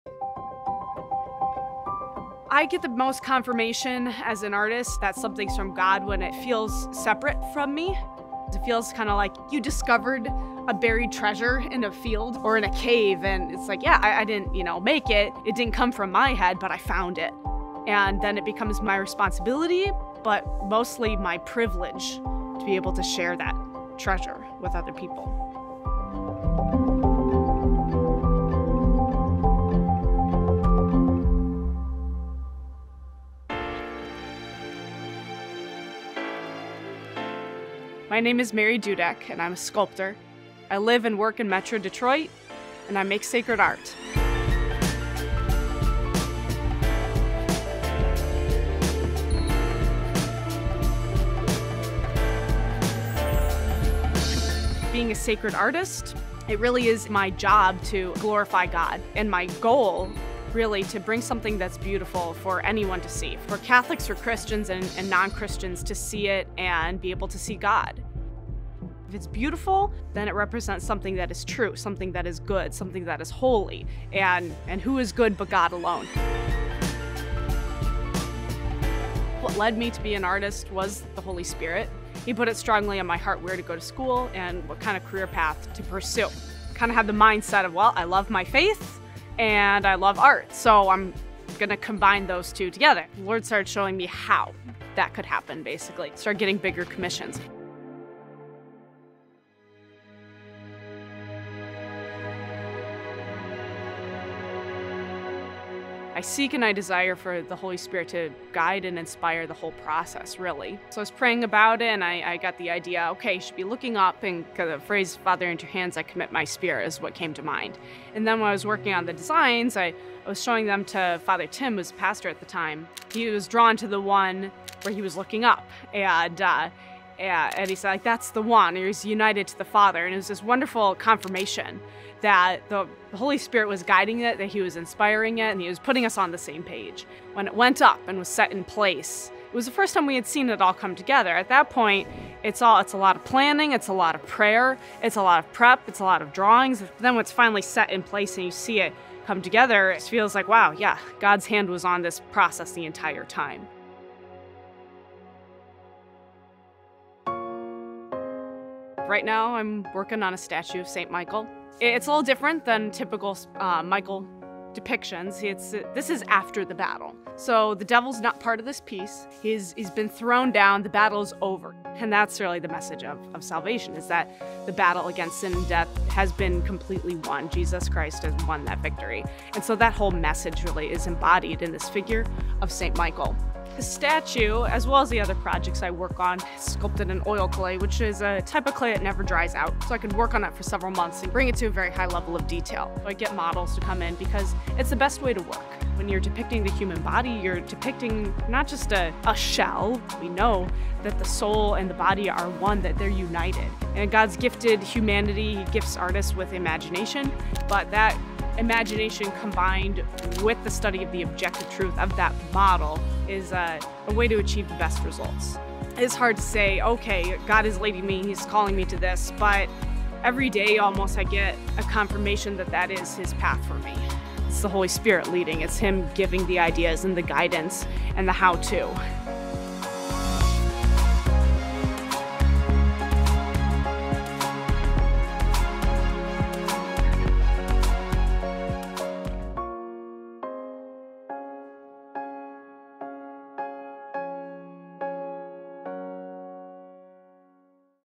Called to Create | Unleashed
Mary Dudek makes beautiful sculptures that she hopes will draw people closer to God. She doesn't see her work simply as the result of her own abilities, but rather as a gift from God led by the Holy Spirit. "Almost every day I get a confirmation that that is his path for me. It's the Holy Spirit leading, it's him giving the ideas and the guidance and the how-to," she shares.

As Marker 4.3 of the Unleash The Gospel Pastoral Letter states, "Christ’s disciples were never meant to carry out her divine mandate to evangelize on the basis of human resources alone. Christ has endowed his bride with an abundance of supernatural charisms, gifts of the Holy Spirit that equip every member of the body of Christ to do his or her part in bearing witness to Christ and building up the Church."

| VIDEO TRANSCRIPT |
I get the most confirmation as an artist that something's from God, when it feels separate from me, it feels kinda like you discovered a buried treasure in a field or in a cave. And it's like, yeah, I didn't, you know, make it, it didn't come from my head, but I found it. And then it becomes my responsibility, but mostly my privilege to be able to share that treasure with other people.

My name is Mary Dudeck and I'm a sculptor. I live and work in Metro Detroit and I make sacred art.

Being a sacred artist, it really is my job to glorify God and my goal, really, to bring something that's beautiful for anyone to see for Catholics or Christians and non-Christians to see it and be able to see God. If it's beautiful, then it represents something that is true, something that is good, something that is holy and, and who is good but God alone? What led me to be an artist was the Holy Spirit. He put it strongly in my heart where to go to school. And what kind of career path to pursue kind of have the mindset of, well, I love my faith and I love art. So I'm going to combine those two together. The Lord started showing me how that could happen and I started getting bigger commissions.

I seek and I desire for the Holy Spirit to guide and inspire the whole process, really. So I was praying about it and I got the idea, okay, should be looking up, and the phrase, "Father into your hands, I commit my spirit," is what came to mind. And then when I was working on the designs, I was showing them to Father Tim who was pastor at the time. He was drawn to the one where he was looking up. And, uh, yeah, and he said like, "that's the one, he was united to the Father". And it was this wonderful confirmation that the holy spirit was guiding it, that he was inspiring it. And he was putting us on the same page. When it went up and was set in place, it was the first time we had seen it all come together. At that point. It's all, it's a lot of planning. It's a lot of prayer. It's a lot of prep. It's a lot of drawings. Then what's finally set in place and you see it come together. It feels like, wow. Yeah, God's hand was on this process the entire time.

Right now I'm working on a statue of Saint Michael. It's a little different than typical Michael depictions. It's, this is after the battle. So the devil is not part of this piece. He's been thrown down, the battle's over. And that's really the message of salvation is that the battle against sin and death has been completely won. Jesus Christ has won that victory. And so that whole message really is embodied in this figure of St. Michael. The statue, as well as the other projects I work on, sculpted in oil clay, which is a type of clay that never dries out. So I could work on that for several months and bring it to a very high level of detail or get models to come in because it's the best way to work.

When you're depicting the human body, you're depicting not just a shell. We know that the soul and the body are one — that they're united. And God has gifted humanity, gifted artists with imagination. But that imagination combined with the study of the objective truth of that model is a way to achieve the best results. It's hard to say, okay, God is leading me and he's calling me to this. But almost every day I get a confirmation that that is his path for me. It's the Holy Spirit leading, it's him giving the ideas and the guidance and the how-to.